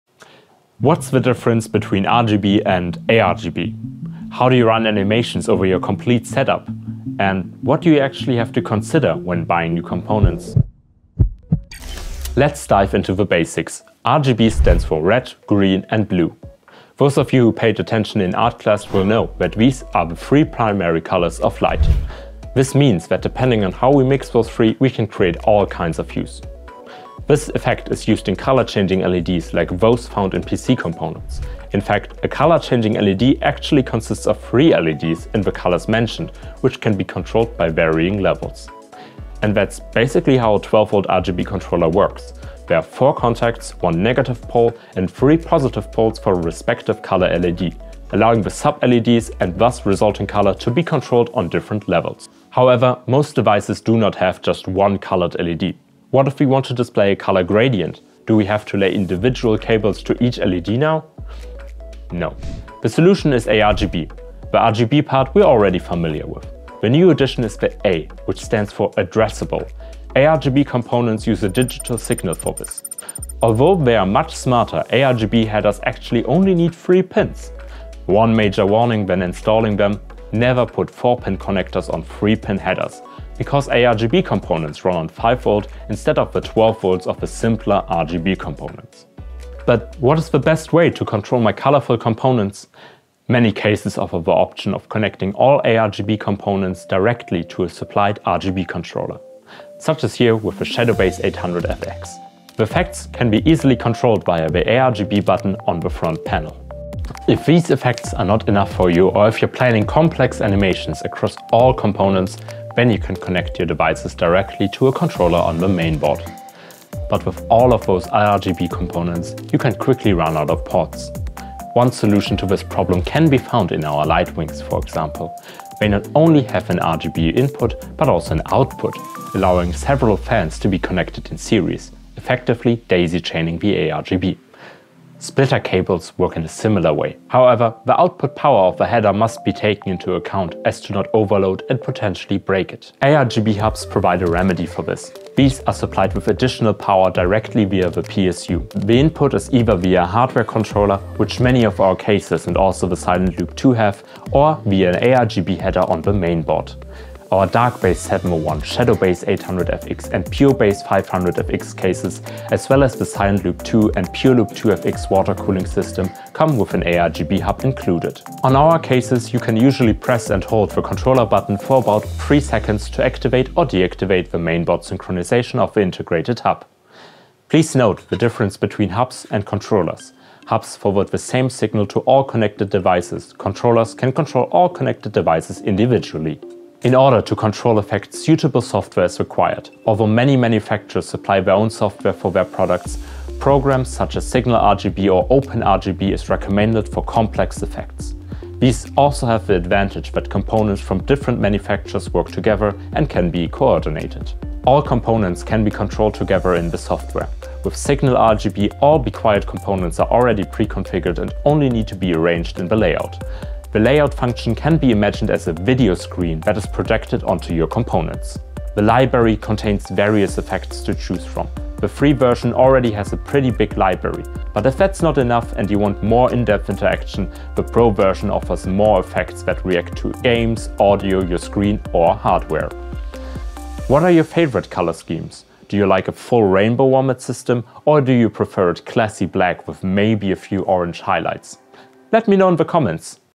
Mastering ARGB Lighting: The Ultimate Guide for PC Gamers | be quiet!
Dive into the world of ARGB lighting with our comprehensive guide. Learn the difference between RGB and ARGB, how to create stunning animations, and what to consider when buying components. Discover tips on effectively controlling your ARGB components, from simple connections to advanced software. Perfect for any PC enthusiast looking to elevate their setup.